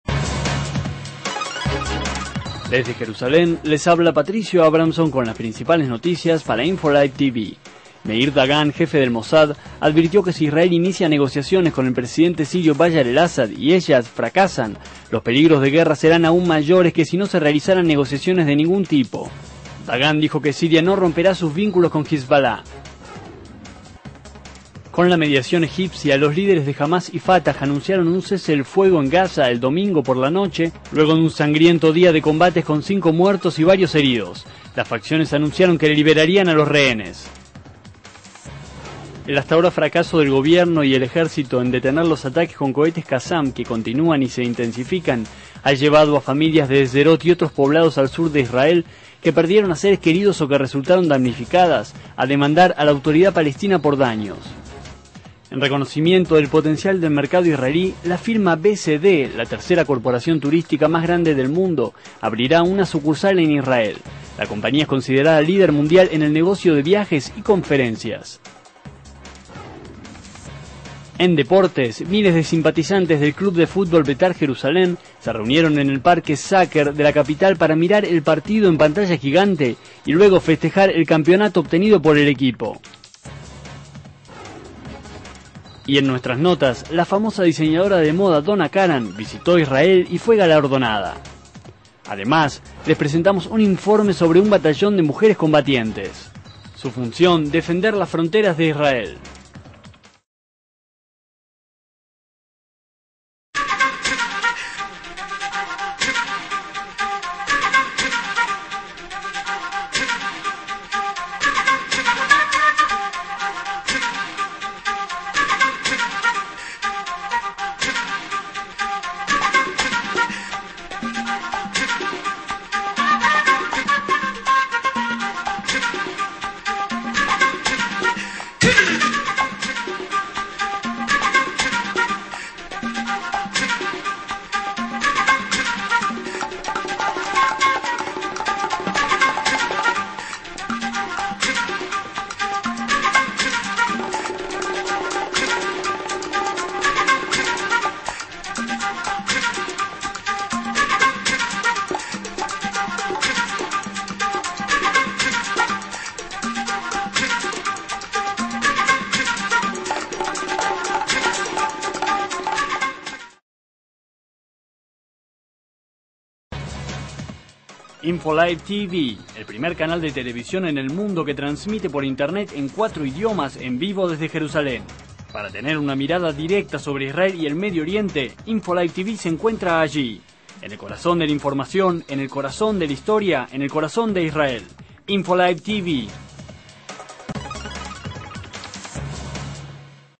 Flash de Información
Los titulares:Advertencias del Mosad.Tregua Fataj - Hamás, luego de sangrientos combates.Familias de Sur demandan a la AP por los kasam.Turismo.Festejos del club Betar Jerusalén.